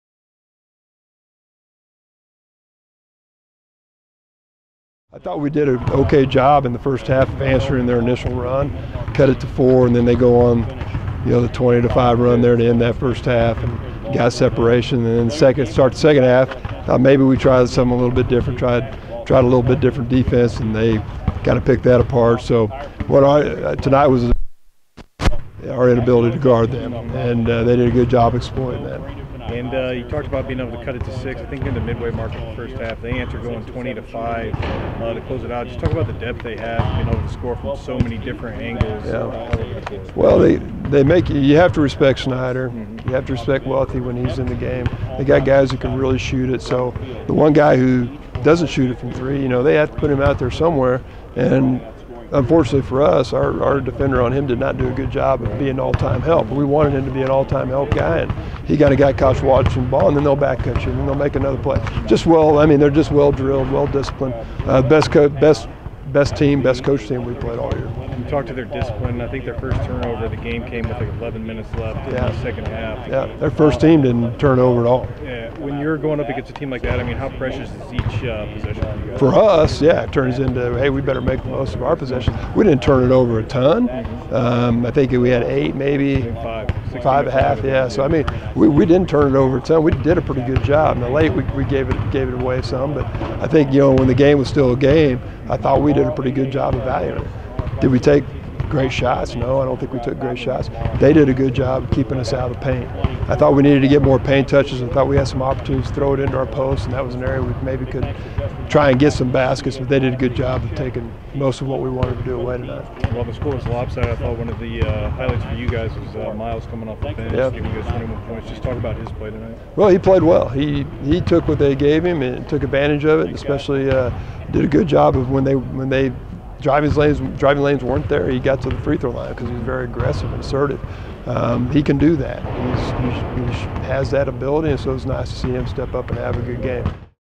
Pitt State Men's Post Game Press Conference - vs #1 Northwest Missouri State
Head Coach Kevin Muff spoke about the disappointing lose to the Bearcats of Northwest Missouri State.